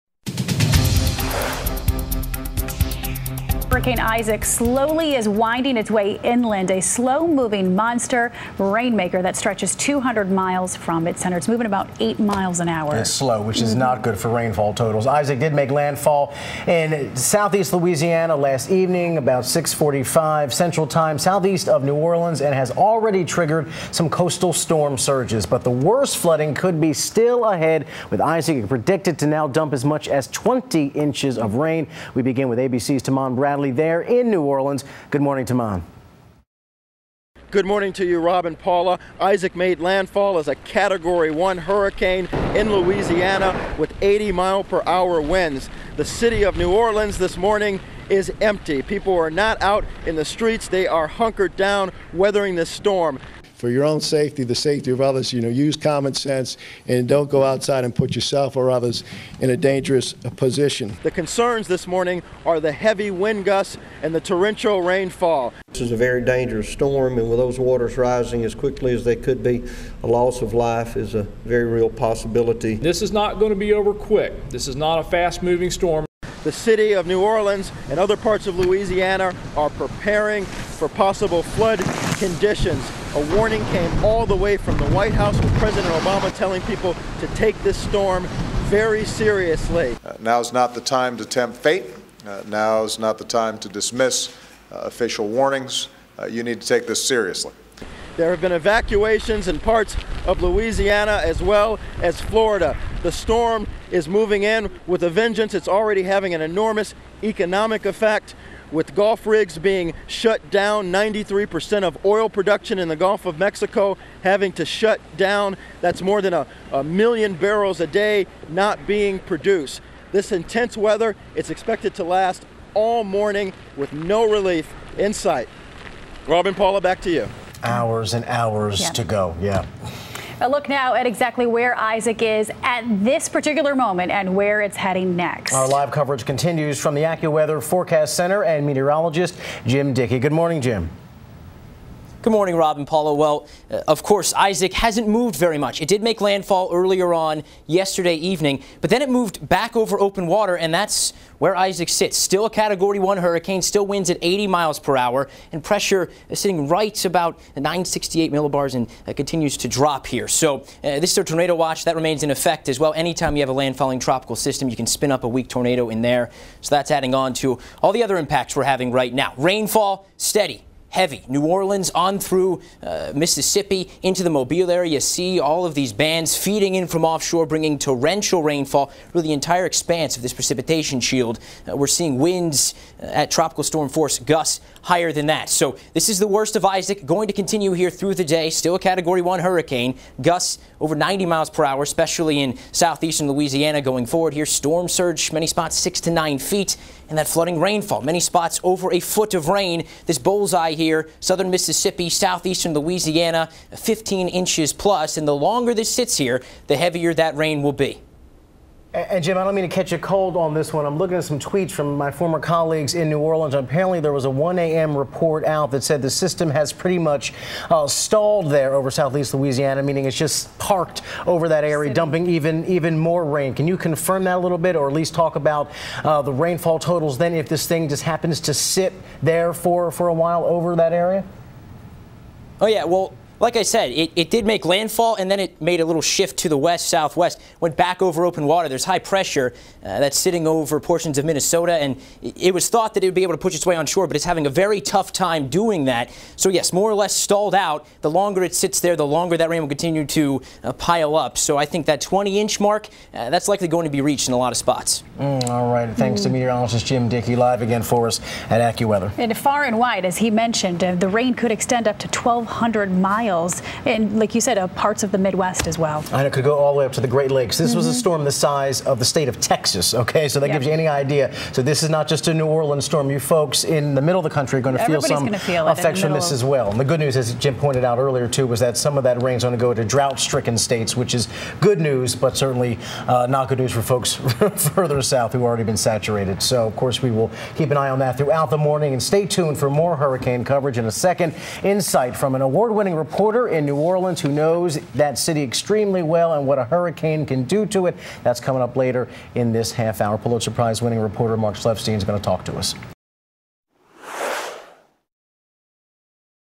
Isaac Expected to Make Second Landfall
More than 300,000 homes and businesses without power in New Orleans.